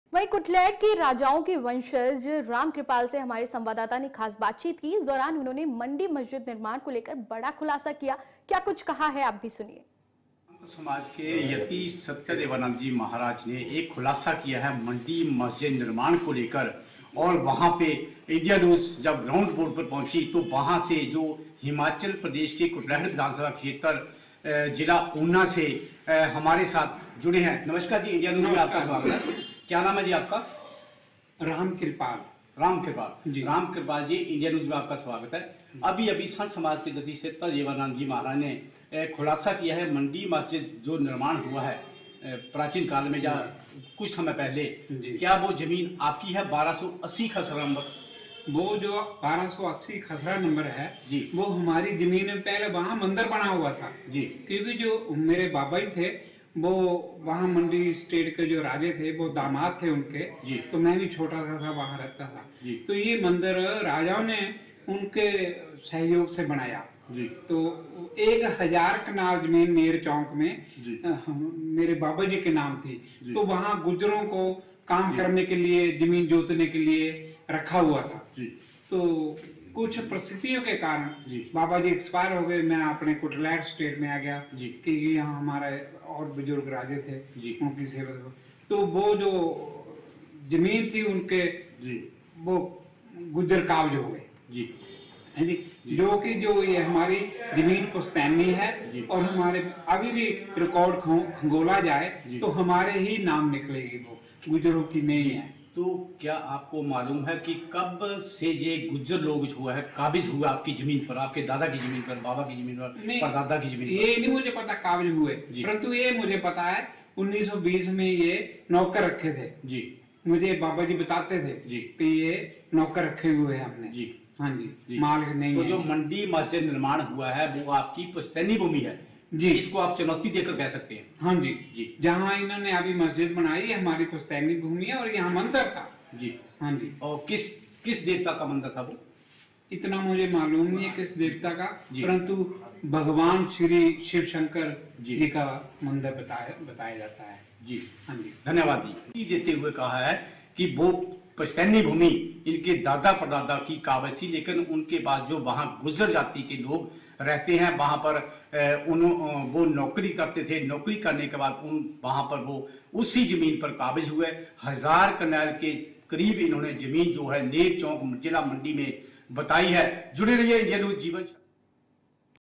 Una : मंडी मस्जिद निर्माण को लेकर किया खुलासा | India News Himachal
कुटलैहड़ के राजाओं के वंशज रामकृपाल से हमारे संवाददाता ने खास बातचीत की... इस दौरान उन्होंने मंडी मस्जिद निर्माण को लेकर बड़ा खुलासा किया